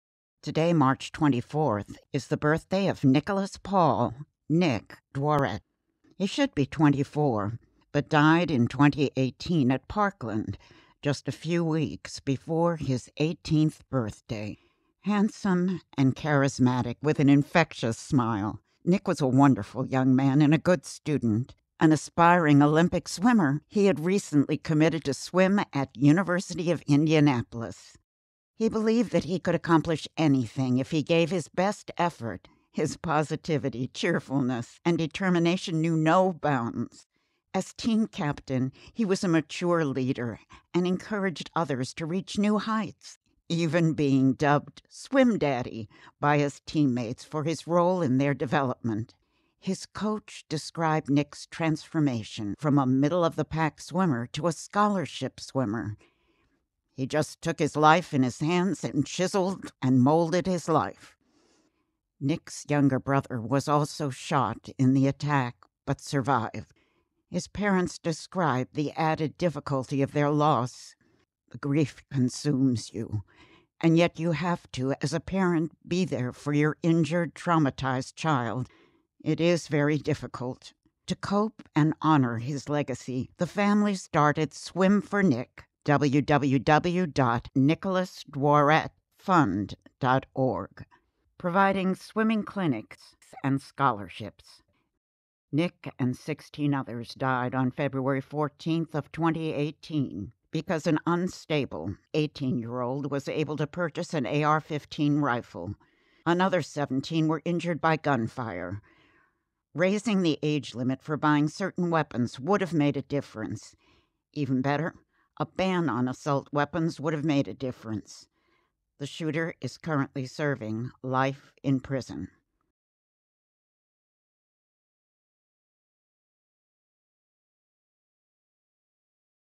Mar 24 Nick Dworet
Today, March 24, is the birthday of Nicholas Paul “Nick” Dworet. He should be 24 but died in 2018 at Parkland just a few weeks before his 18th birthday. Handsome & charismatic with an infectious smile, Nick was a wonderful young man and a good student.  An aspiring Olympic swimmer, he had recently committed to swim at University of Indianapolis.
He believed that he could accomplishing anything if he gave his best effort – his positivity, cheerfulness, and determination knew no bounds. As team captain, he was a mature leader and encouraged others to reach new heights …even being dubbed “Swim Daddy” by his teammates for his role in their development. His coach described Nick’s transformation from a middle-of-the-pack swimmer to a scholarship swimmer: “He just took his life in his hands and chiseled and molded his life.”
Nick’s younger brother was also shot in the attack but survived. His parents described the added difficulty of their loss: “The grief consumes you and yet you have to, as a parent, be there for your injured, traumatized child, it is very difficult.” To cope and honor his legacy, the family started Swim 4 Nick providing swimming clinics and scholarships.
Nick and 16 others died on February 14th of 2018 because an unstable 18-year-old was able to purchase an AR-15 rifle. Another 17 were injured by gunfire. Raising the age limit for purchasing certain weapons would have made a difference. Even better, a ban on assault weapons would have made a difference. Despite his callous attitude toward his victims, the shooter was granted life in prison rather than the death penalty.